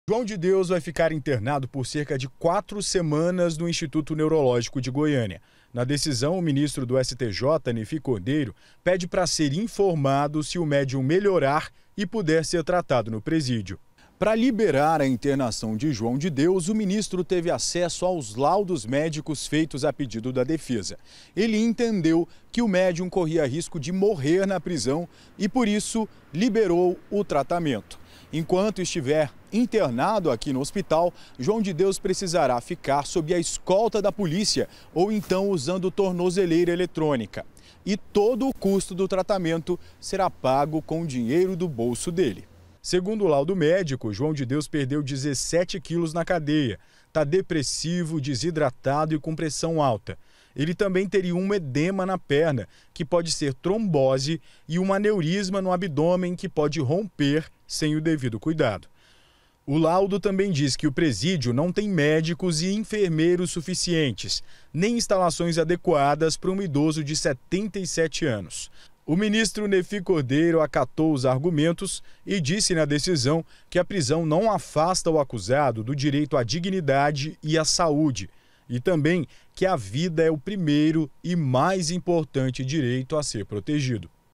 DECISÃO DO STJ: JOÃO DE DEUS DEVE SER LIBERADO HOJE
O médium João de Deus deve chegar a qualquer momento no Hospital Neurológico de Goiânia para fazer um tratamento. A decisão foi autorizada pelo ministro do Superior Tribunal de Justiça, Nefi Cordeiro.